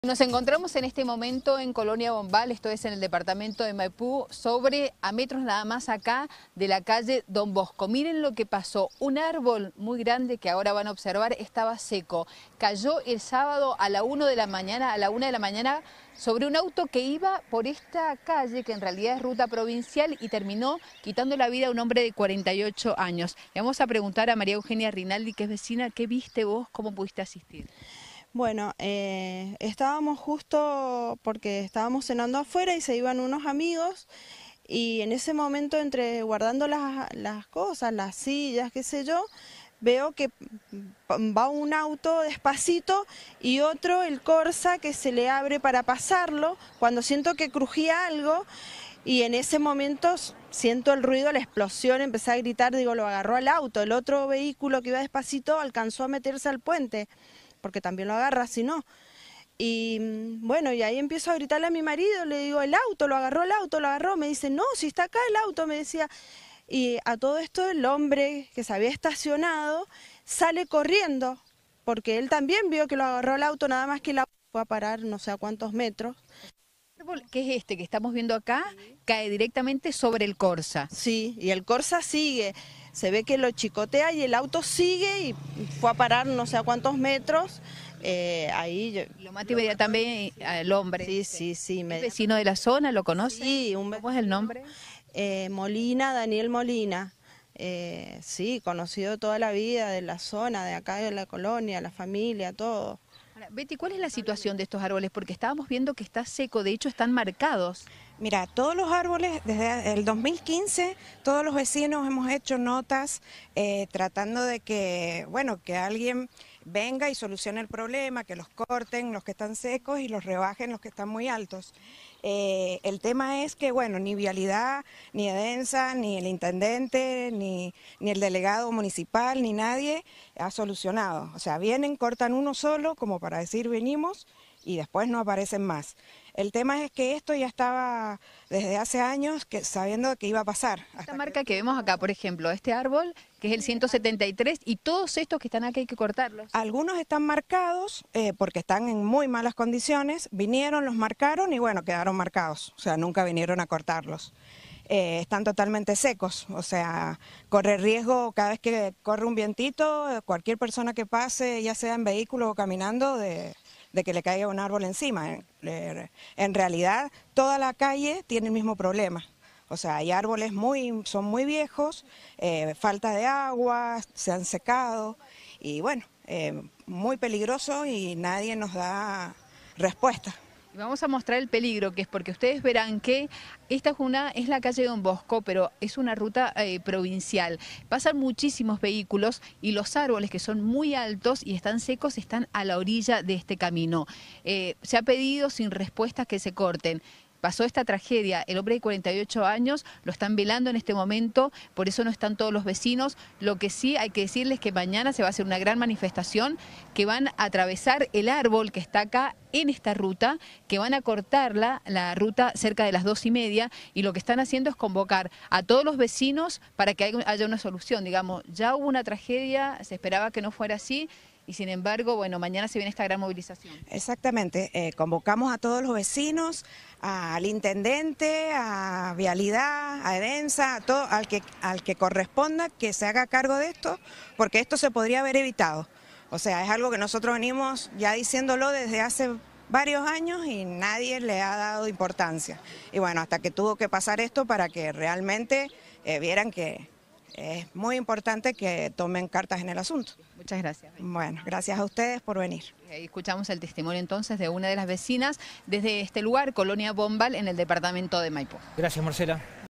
MURIÓ UN HOMBRE APLASTADO POR UN ÁRBOL
Un hombre murió aplastado por un árbol el domingo por la madrugada en Maipú. El testimonio de los vecinos del lugar.